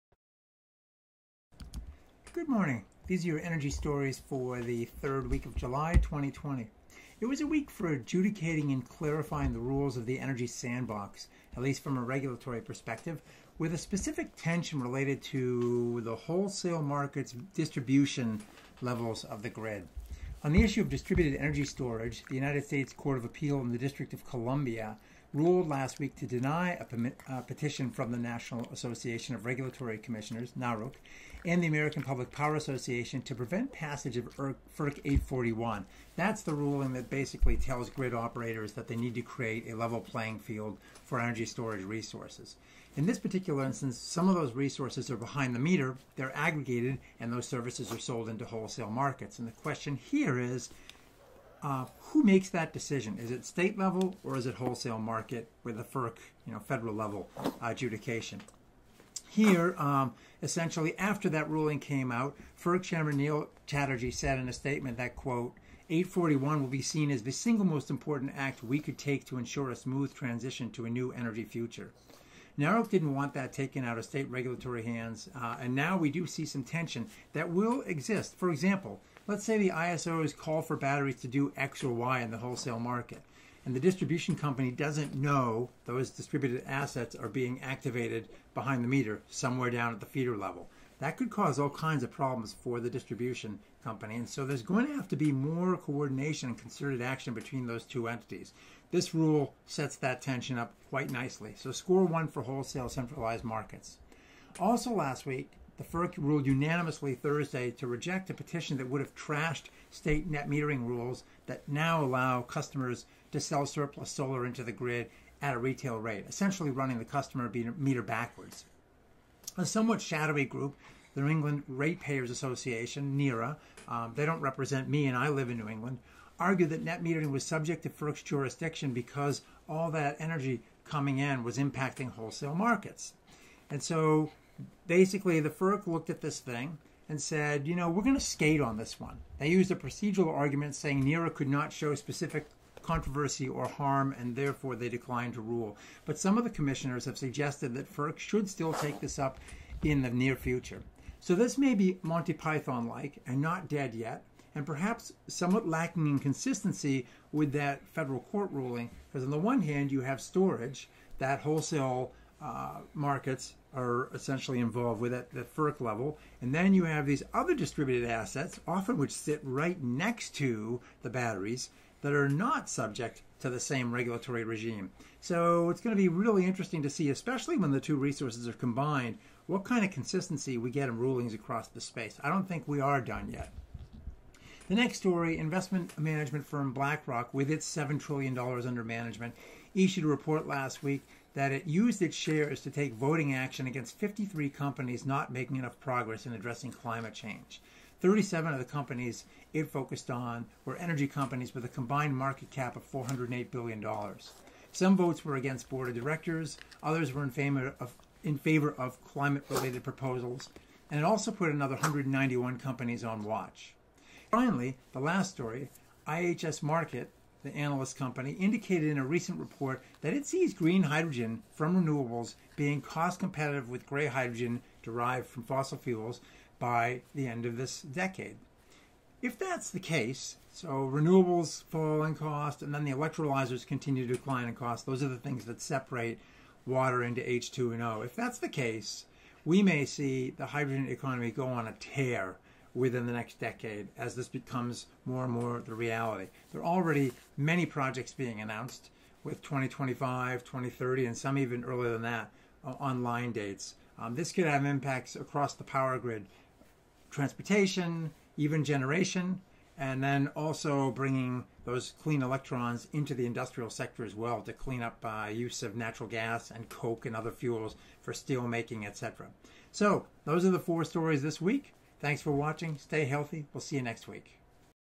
Distributed Energy Storage Affirmed by US Court of Appeals
Distributed Energy Storage Affirmed by US Court of Appeals; Solar Net Metering Challenge Rejected by FERC; BlackRock Fires Climate Warning Shots; and Green H2 Competitive by 2030

1) US D.C. Court of Appeals denied a NARUC and APPA petition to halt passage of FERC 841. As a result, on-site storage can participate in wholesale markets. Tension is now likely to arise between those markets and the distribution utilities that don't know when those distributed assets are activated, which could cause problems.

2) The FERC ruled unanimously to reject a petition that would have quashed state net metering rules allowing customers to sell surplus solar into the grid at a retail rate, essentially running the customer meter backwards. FERC used a procedural argument to say no, so this may recur.

3) Investment management firm BlackRock - with its $7 trillion under management - reported last week that it used its shares to vote against 53 companies not making progress in addressing climate change. 37 of the companies it addressed were energy companies with the combined market cap of $408 billion.

4) IHS Markit says green hydrogen from renewables being cost competitive with fossil fuel-derived gray hydrogen by 2030.